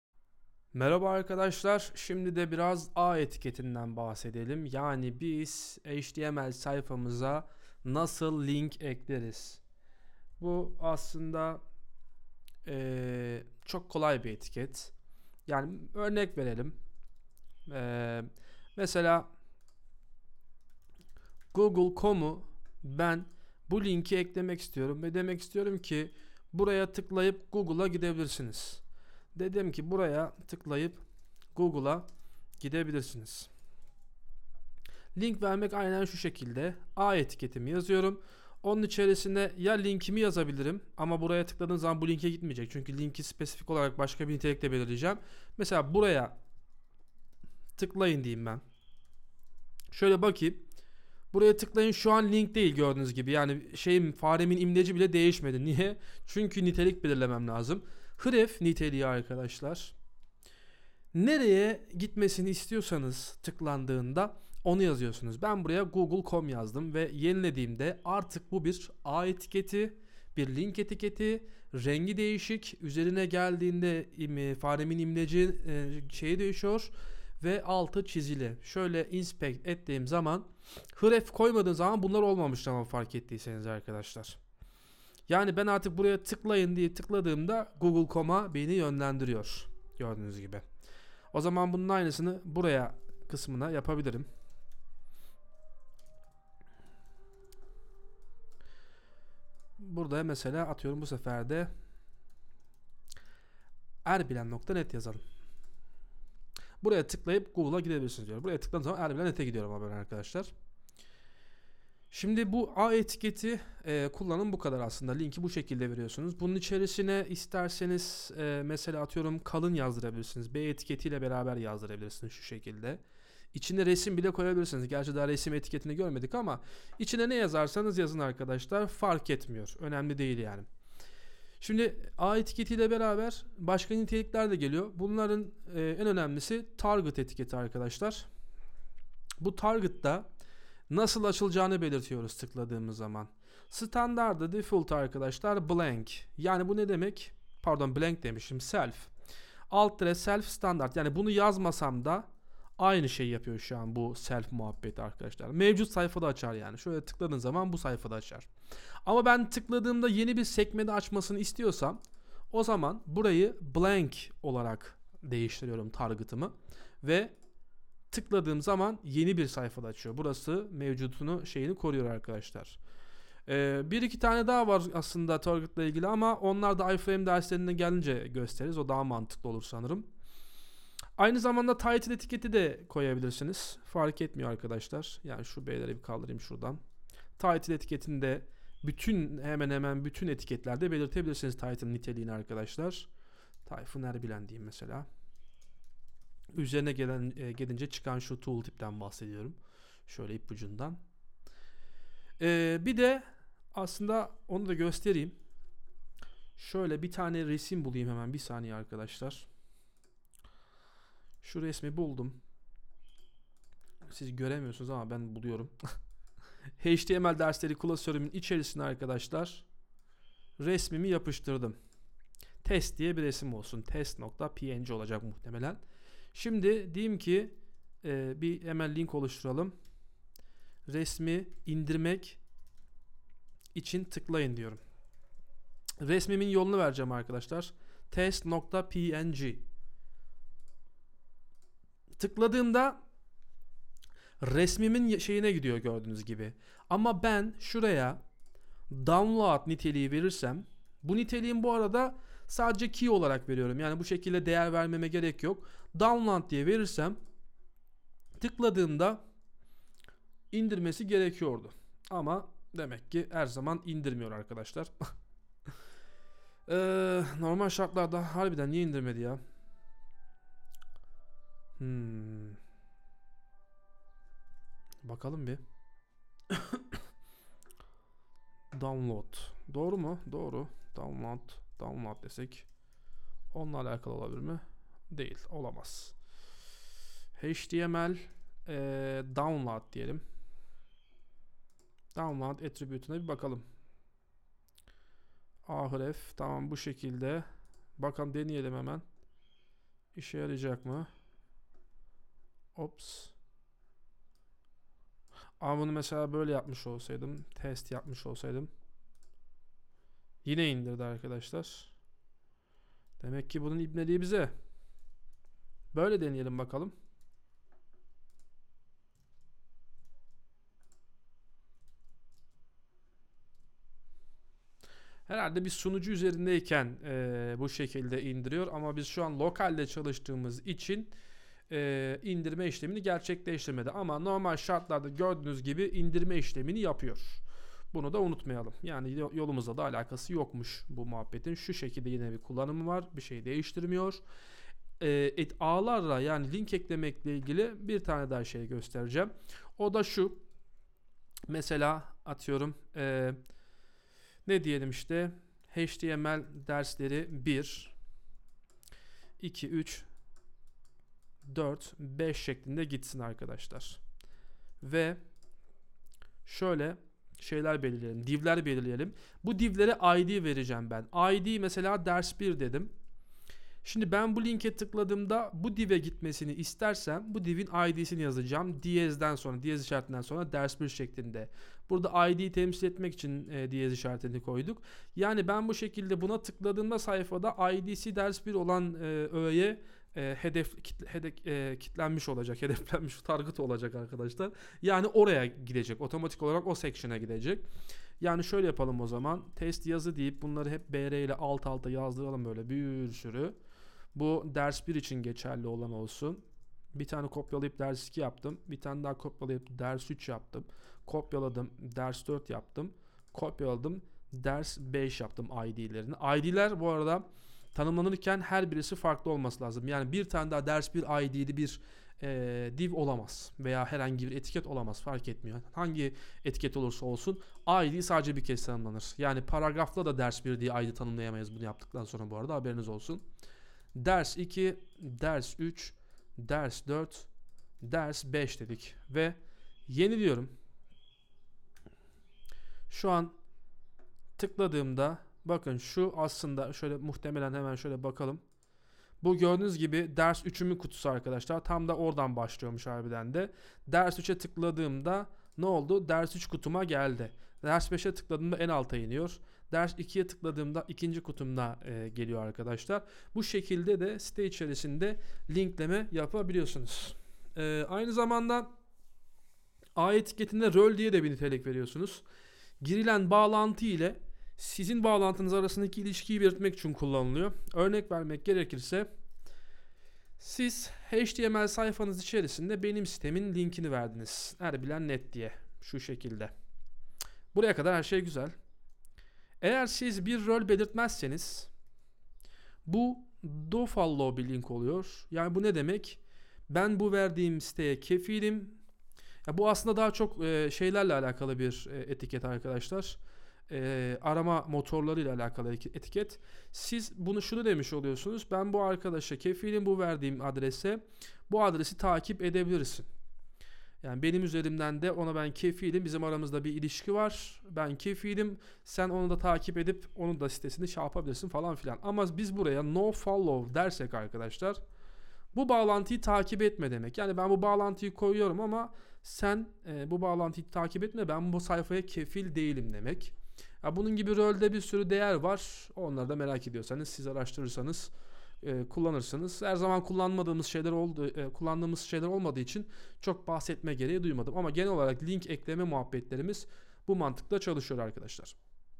HTML Dersleri -7 / Link Etiketi
Bu ders Tayfun Erbilen tarafından hazırlanmıştır.

HTML dersleri serimizin yedinci bölümünde, link etiketi olan a etiketini detaylı bir şekilde ele alacağız. Bu videoda, başka sayfalara, e-posta adreslerine ve harici kaynaklara bağlantılar oluşturmanın nasıl yapıldığını öğreneceksiniz. Ayrıca, bağlantıların hedef özelliklerini ve stil seçeneklerini nasıl yöneteceğinizi de göreceksiniz. Bu bölüm, kullanıcıların web sayfalarınız arasında gezinmesini sağlamak ve kaynaklarınıza kolay erişim sağlamak için HTML link etiketinin kullanımını anlamanıza yardımcı olacaktır.

Yeni çıkardığım PHP Eğitim Setime gözatın!